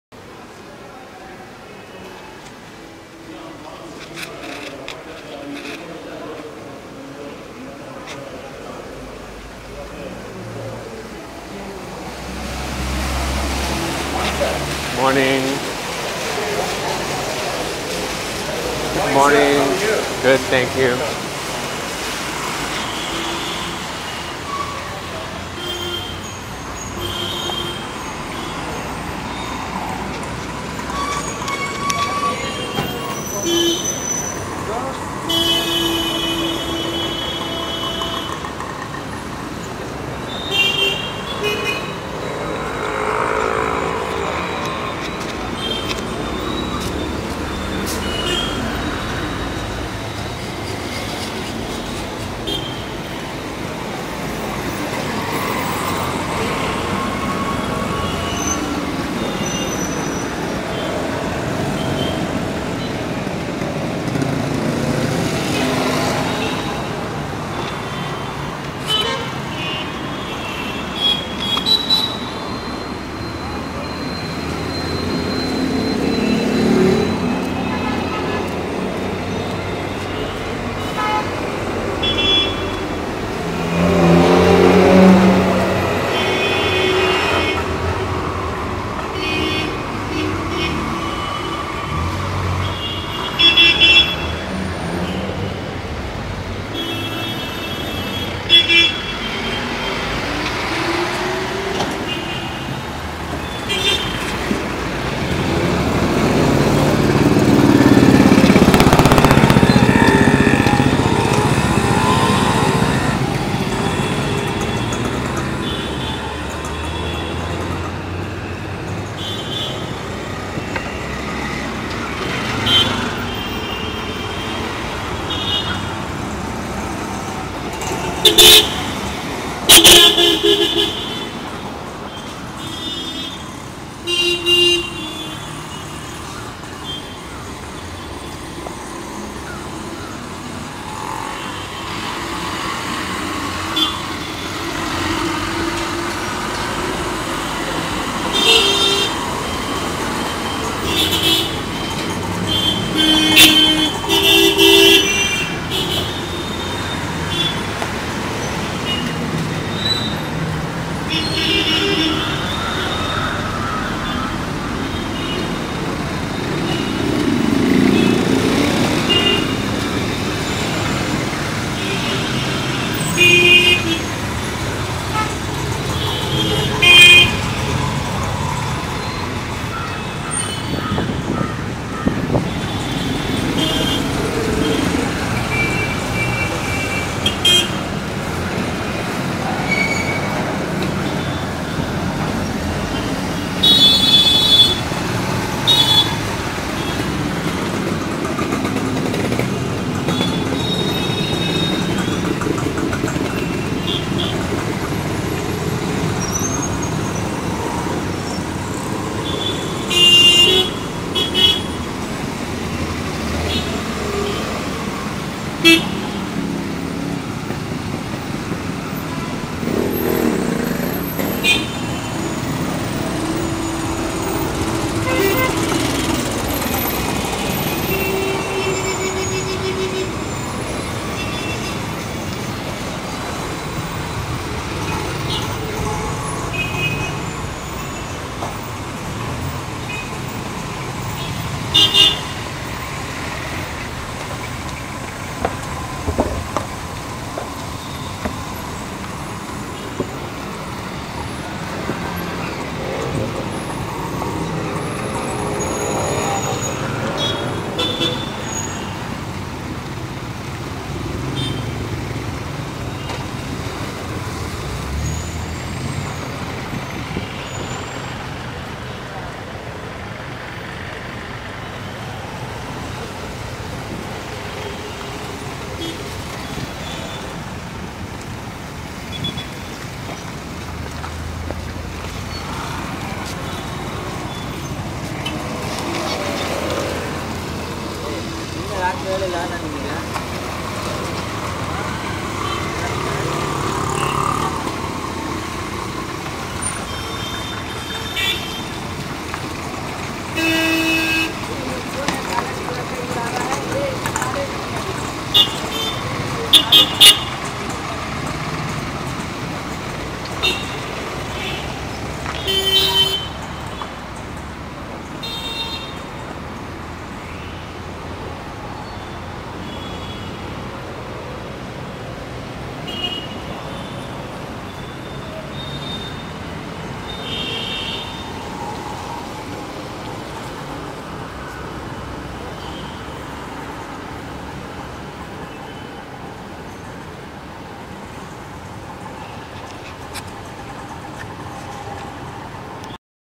Walk to work in bangalore
A simple test of the iPod Nano video camera. My walk in the morning from the Taj Residency to the Obopay office in Bangalore. Down M.G. Boulevard during the construction of the overhead train.